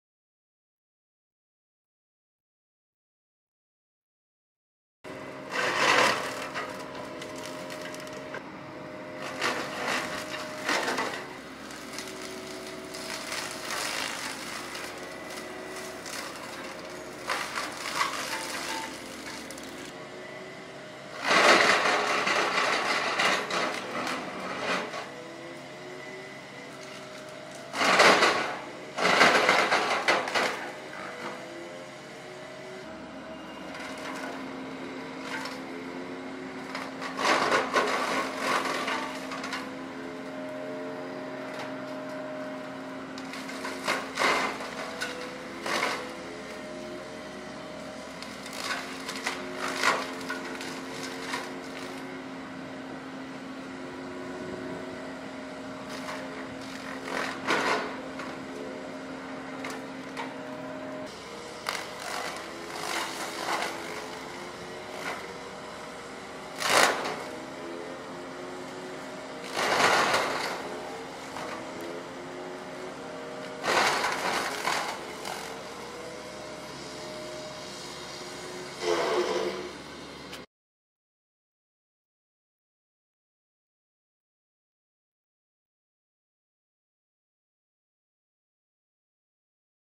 Mini Ex Piston Brush Cutter | Diamond Tool Store
Blue Diamond® Mini Excavator Severe Duty Brush Cutter is a serious attachment for clearing land!